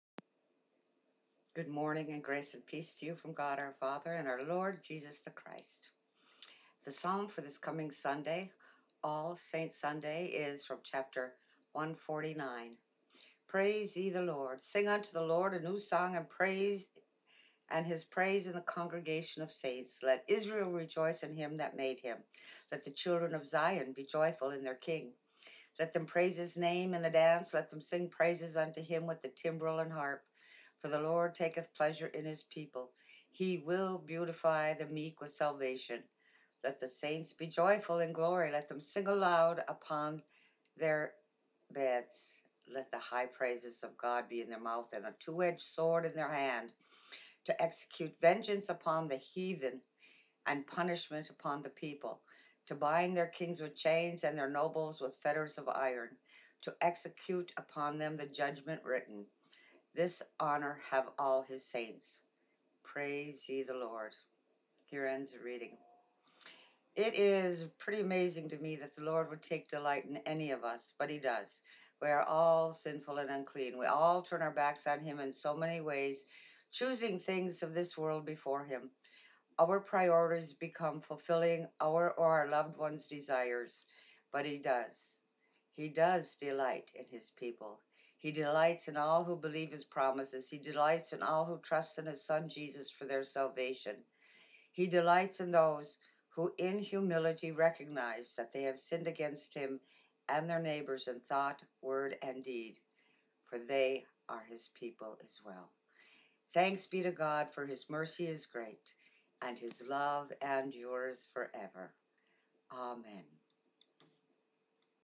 The Lord Delights In His People!
Psalm 149 KJV; All Saints Sunday; In spite of being sinners; the Lord still delights in us.